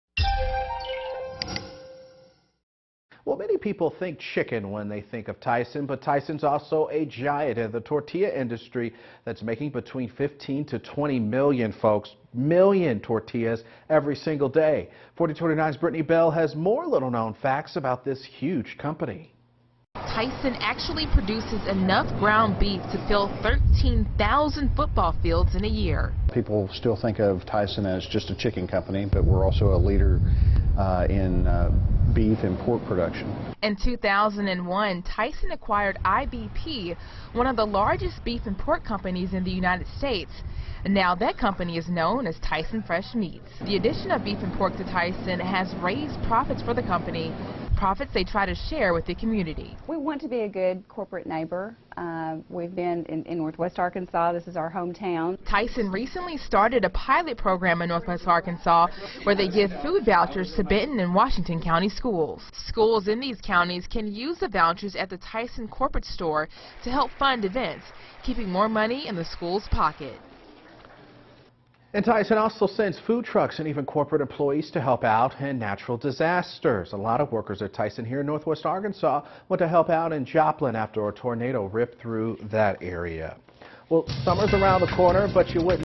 Tyson Foods big business in other food industries besides chicken and gives back communities
Consumers associate Tyson Foods with chicken, but it has many hands in other big businesses including beef and tortillas. Tyson also is spends time and money giving back to the local community.